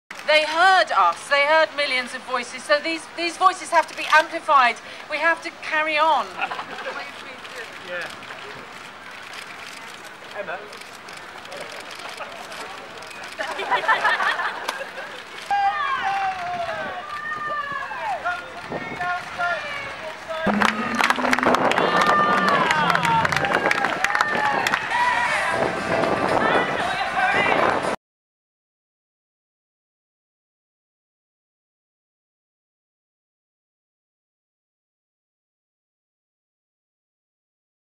Emma Thompson joins Greenpeace celebrations in London | Newzulu Video of the Day
Actress Emma Thompson, who has been outspoken against Shell's Arctic drilling programme, joined Greenpeace members, including Director John Sauven, and members of the public on September 29, 2015 to celebrate Shell's announcement that it was dropping its plans to drill for oil off the Alaskan coast.

By Jameswalter

Share your news video. Get published, get paid.